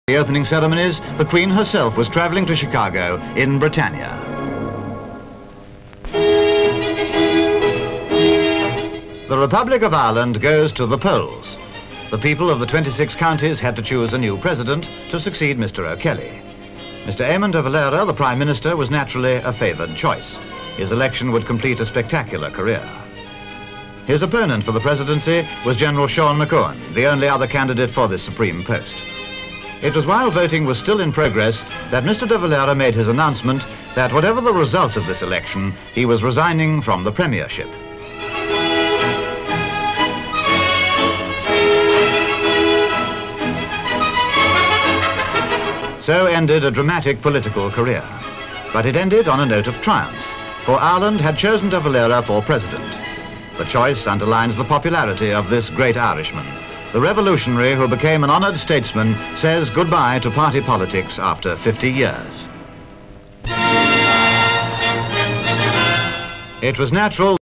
IRISH ELECTION
(22 Jun 1959) The Republic of Ireland went to the Polls. The people of the 26 Counties had to choose a new President to succeed Mr O'Kelly. Mr Eamon de Valera, the Prime Minister, was a favoured choice and his opponent for the Presidency General Sean Mac Eoin, the only other candidate for this supreme post. But Ireland had chosen de Valera for President.